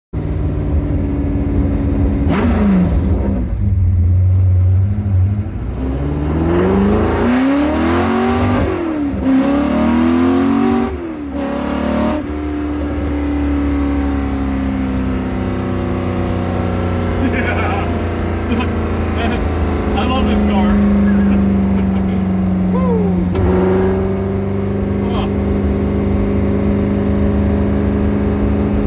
yeeeehaaawwww 🤠 GT40 quick drive
Yeeeeeehaaawwwwww 🤠 this car genuinely just hits right, any bad day after driving it turns into a good one

1966 GT40 MKii Roush 427

If you're interested in your own GT40 shoot me a msg 🦅🇺🇸🍻